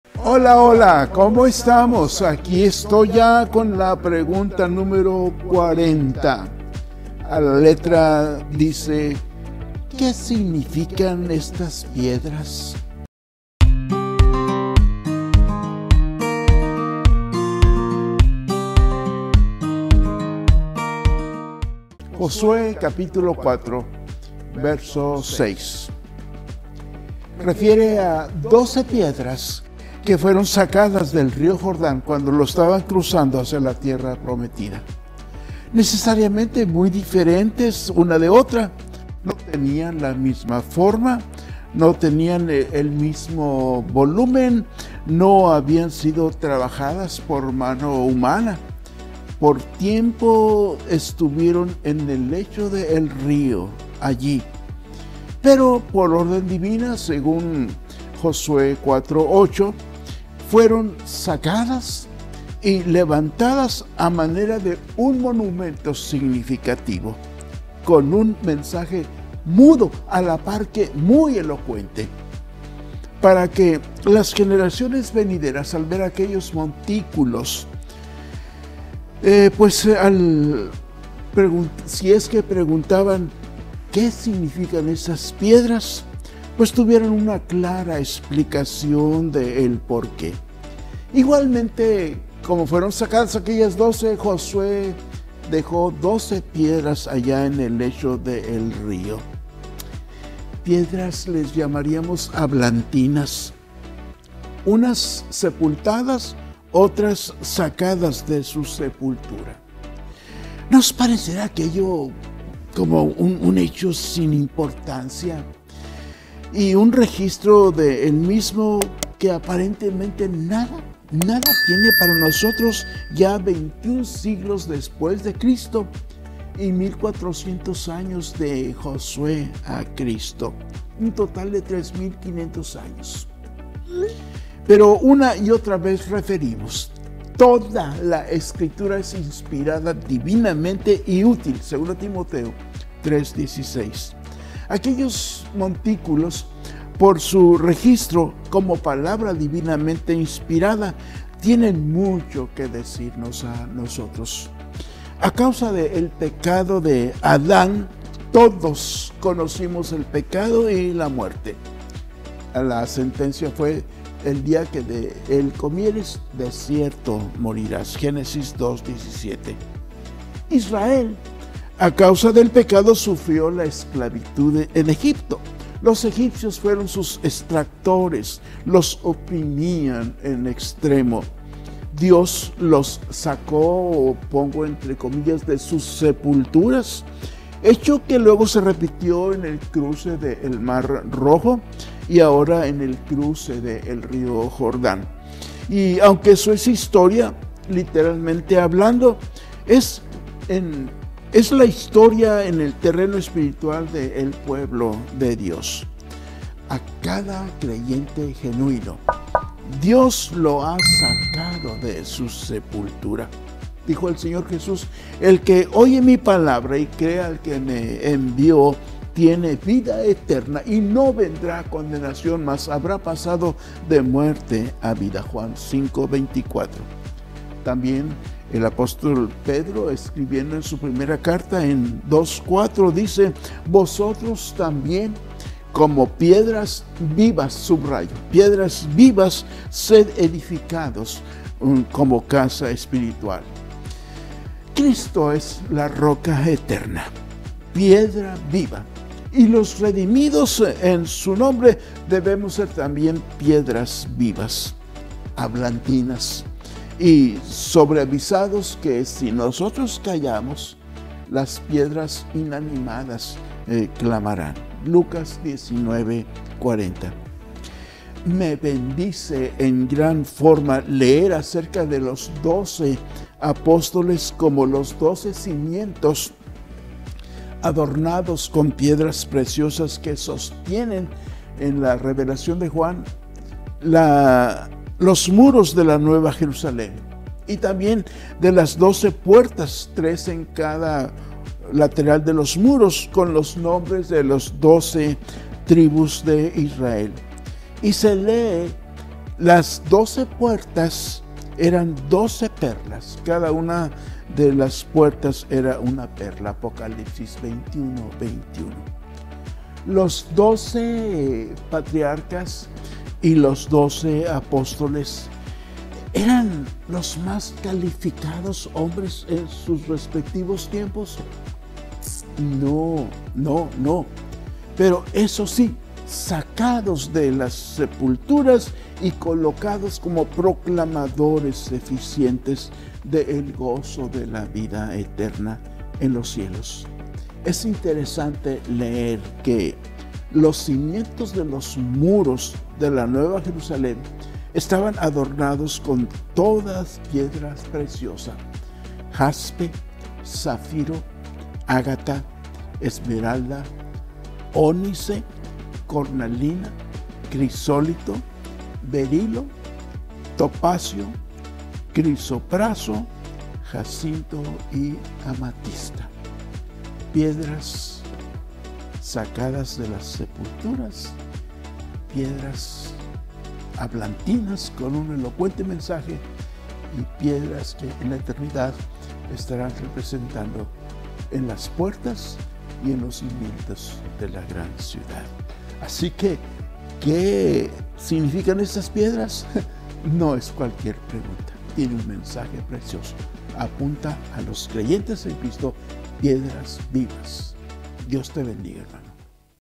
QUE SIGNIFICAN ESTAS PIEDRAS? Josue 4:6 | Pregunta #40
El pastor Emilio Castillo nos comparte la pregunta del dia de hoy en esta nueva serie anual de preguntas biblicas diarias!  Acompañanos a estudiarlo!

Iglesia Bautista Trinity
Pastor: Emilio Castillo

Produced by
ROCA FUERTE STUDIOS
Brownsville, TX.
CEO: Albert Castillo